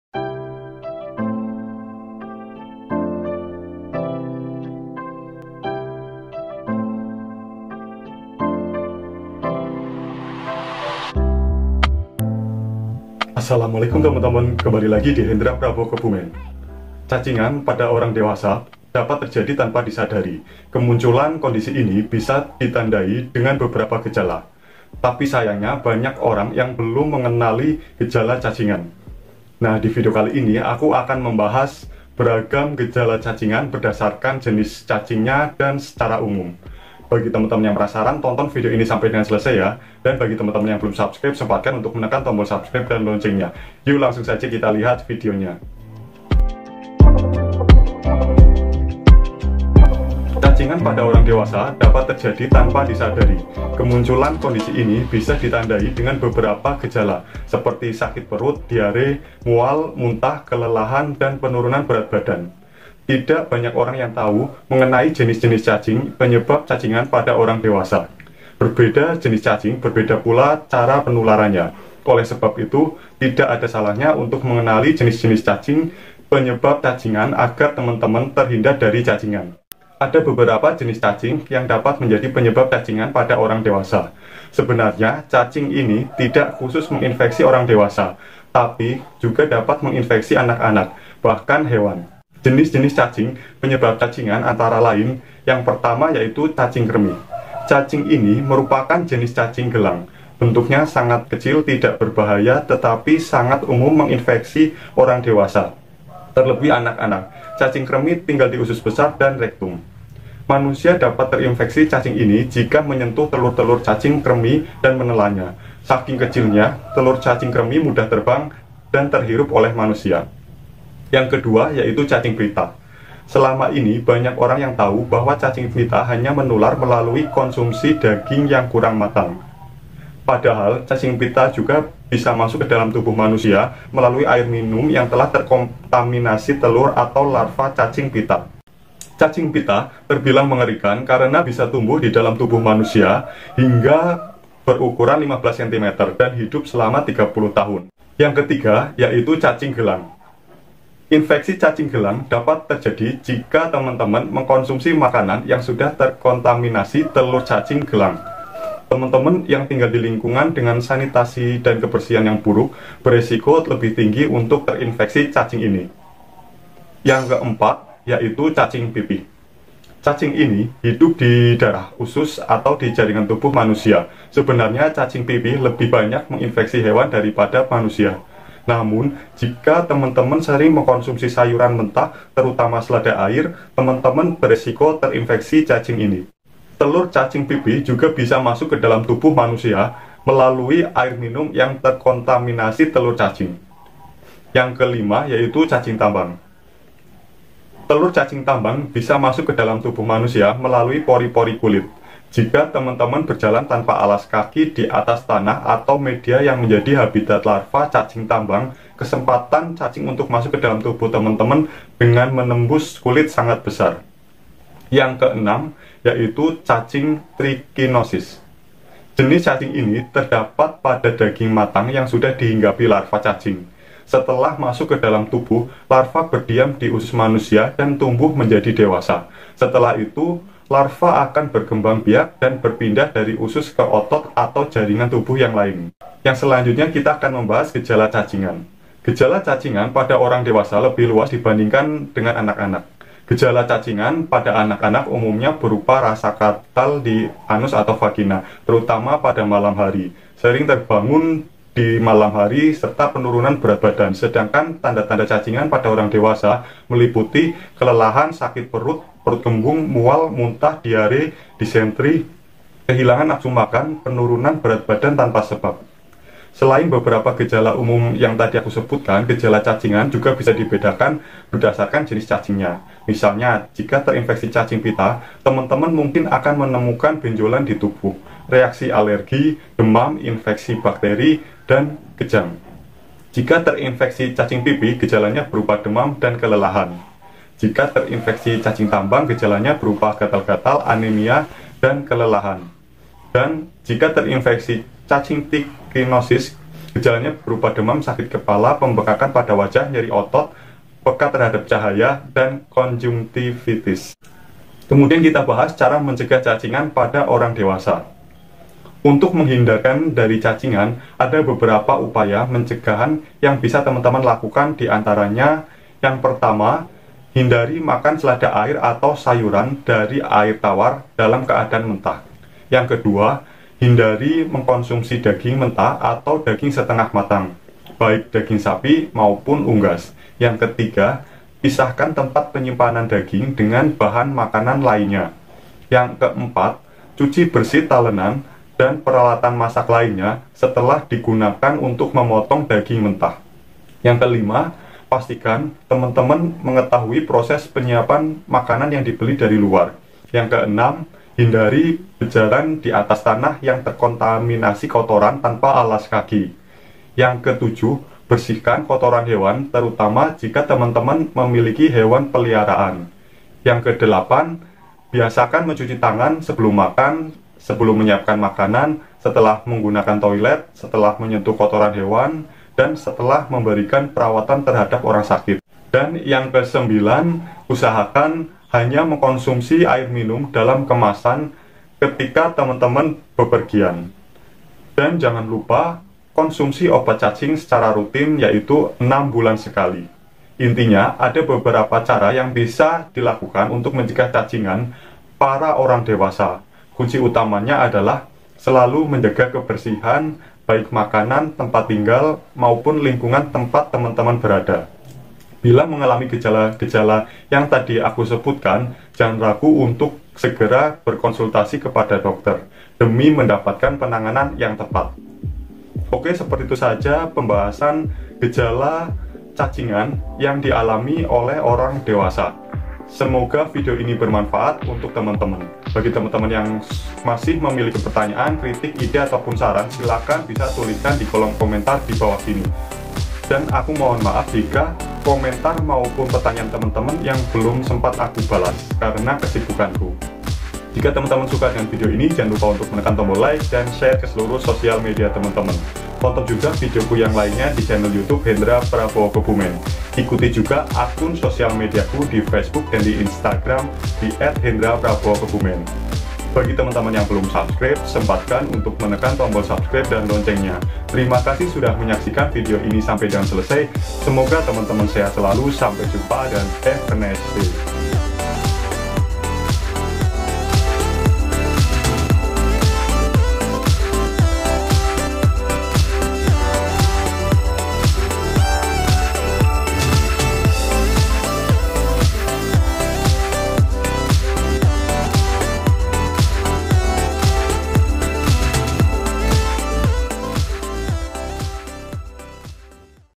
KENALI GEJALA CACINGAN SEJAK DINI PADA ORANG DEWASA || BANYAK ORANG YG TIDAK TAHU GEJALA CACINGAN
Cacingan pada orang dewasa dapat terjadi tanpa disadari. Kemunculan kondisi ini bisa ditandai dengan beberapa gejala. Oleh sebab itu, tidak ada salahnya untuk mengenali jenis-jenis cacing penyebab cacingan dan gejalanya agar terhindar dari cacingan.Biar gak penasaran yuk langsung aja lihat videonya.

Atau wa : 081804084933

Terimakasih,
hendraprabowokebumen